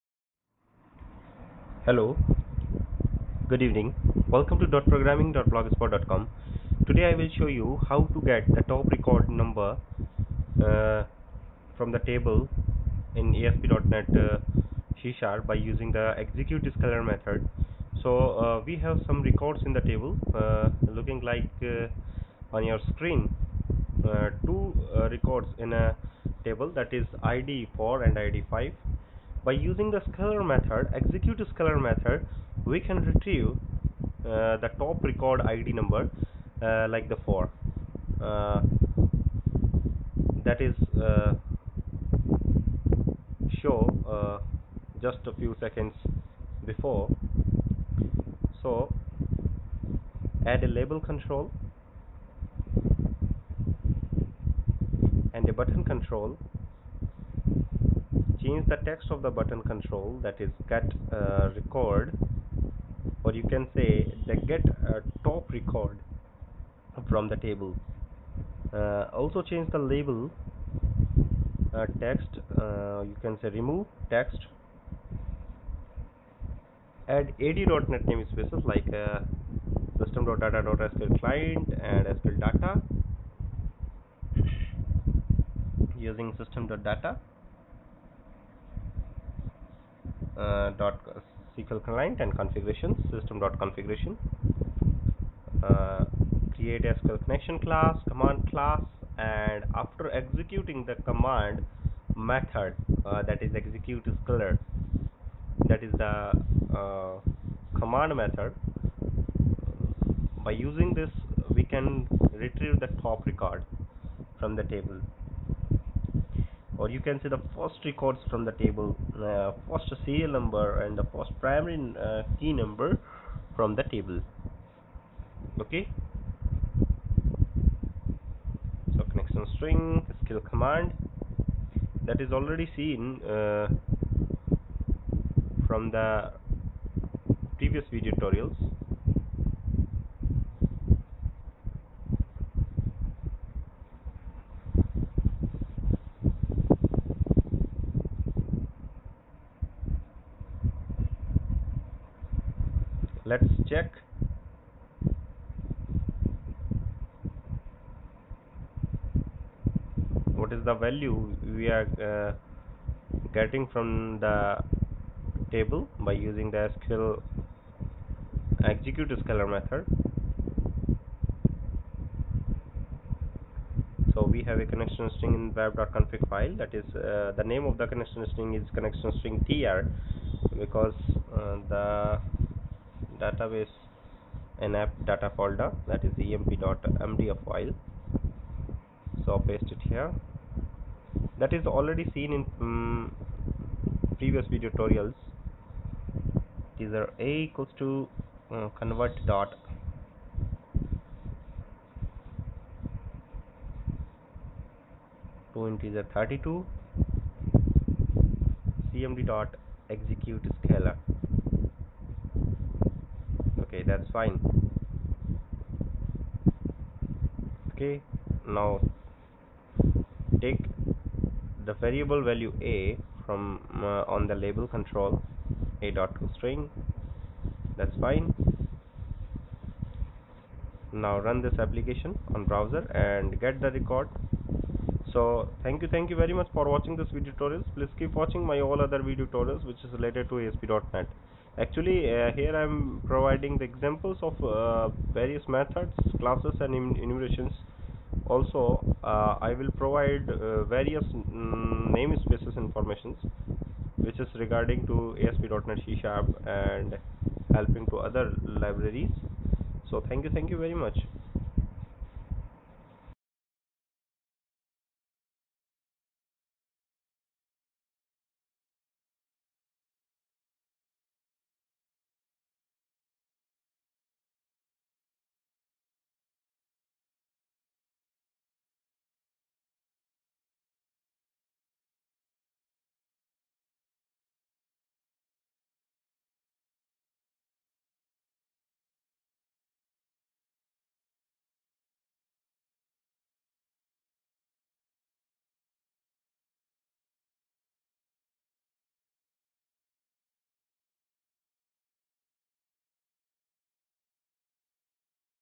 Get TopRecord number using ExecuteScalar method in ASP.NET
In this article i will show you, How to use ExecuteScalar( ) method in ASP.NET C#.
--How to get the top record from database using Execute scalar method.